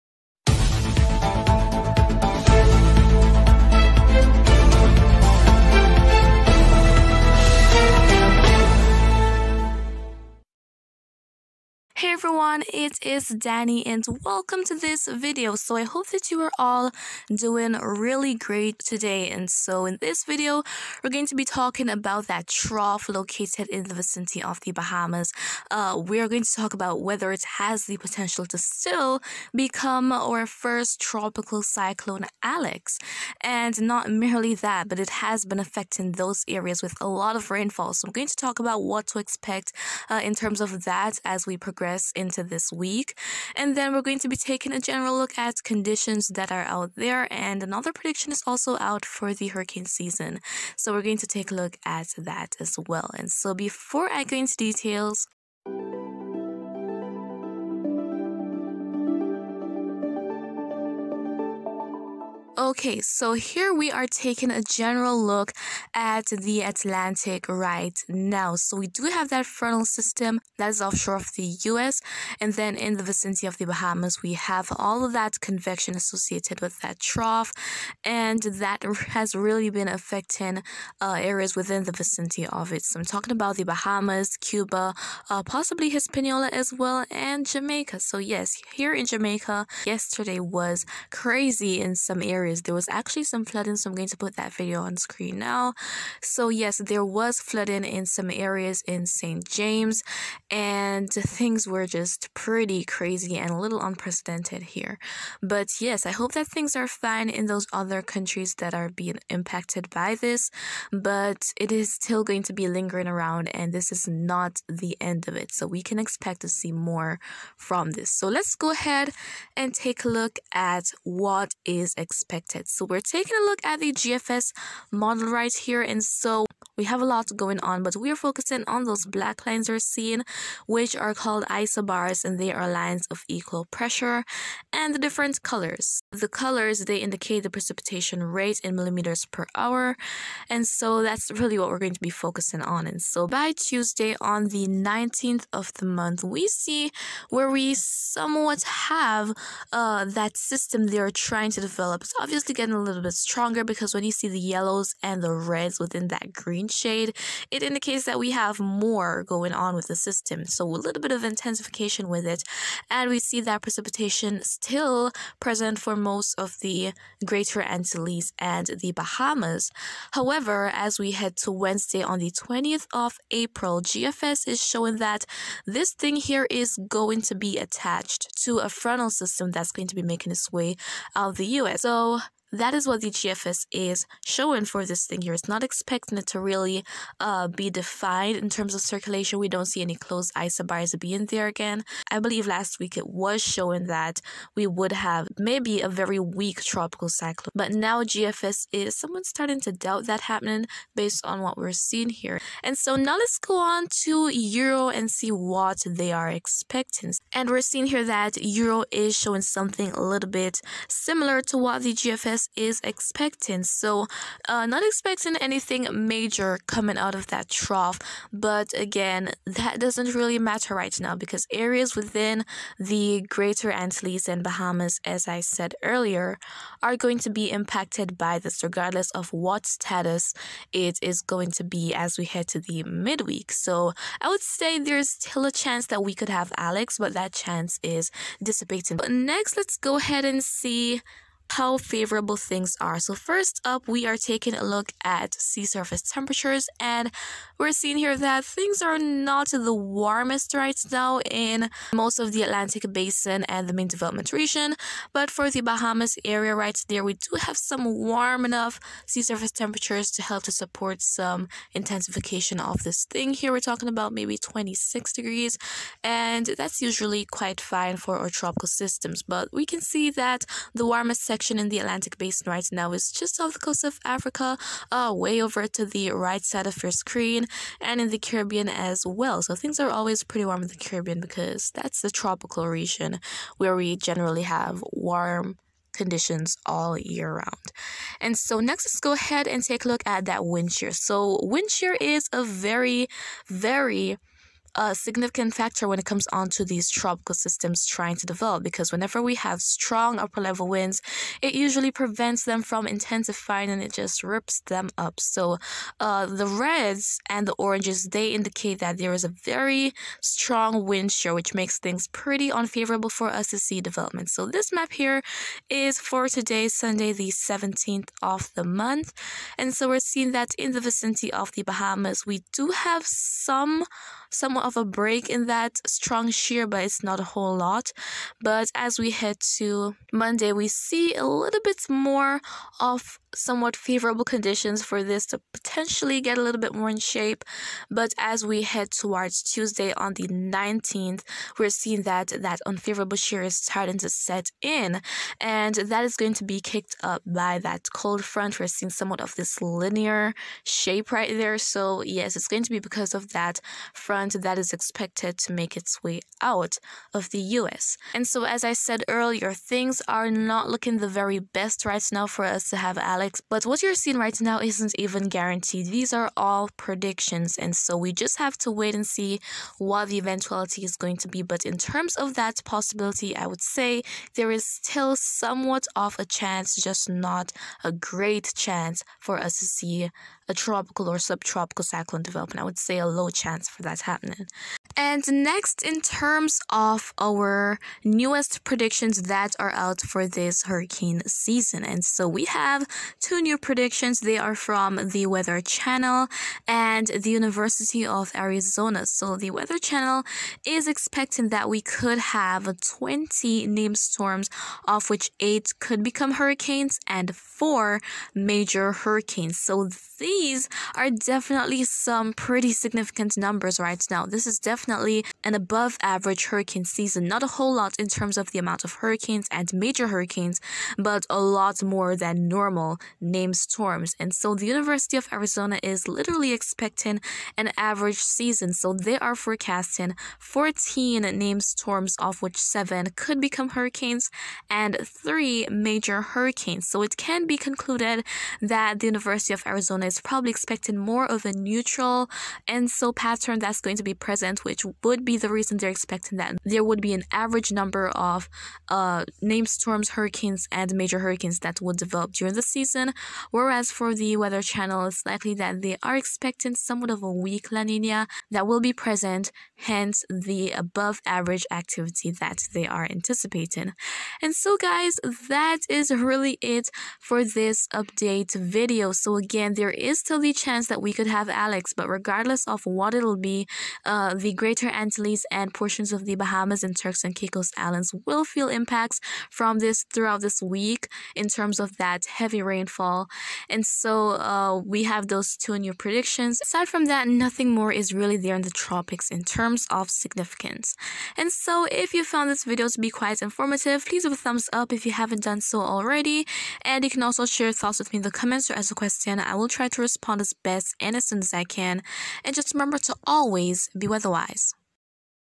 (Sub)tropical storm ALEX still possible? • Heavy rainfall to affect the Caribbean
A trough lingers in the vicinity of the Bahamas which is resulting in heavy rainfall across areas within the vicinity but what is the possibility of this advancing into a cyclone? This video explains all you need to know.

~weather.gurl.danii